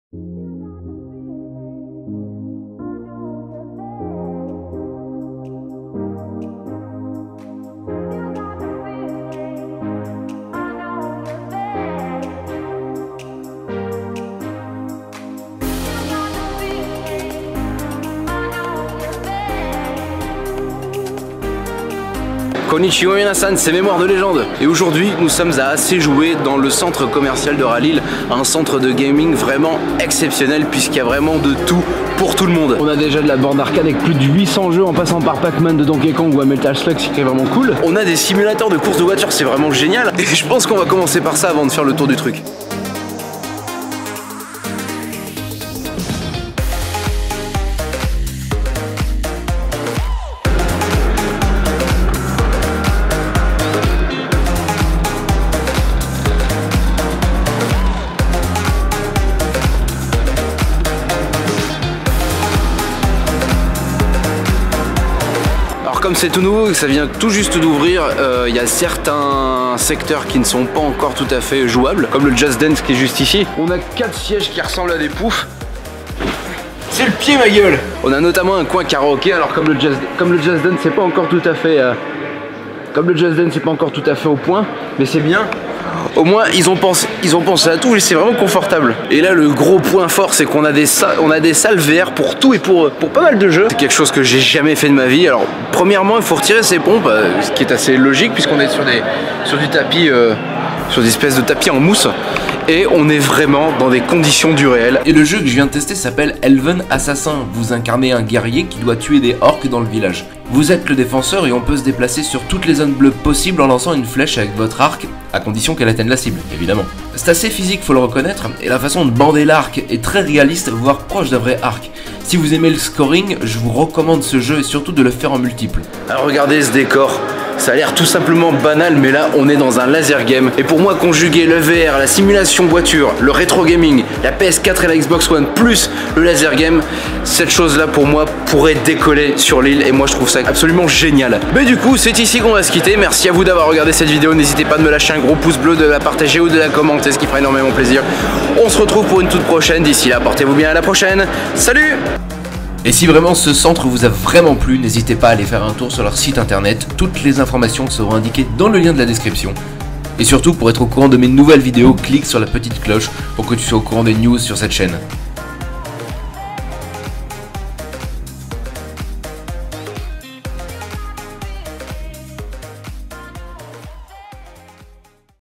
UN CENTRE DE GAMING A LILLE
Vous avez des enfants et ils s'ennuient au centre commercial ?
Emmenez les à Assez joué le temps de votre promenade .

● INFORMATION ●

● ME SUIVRE ●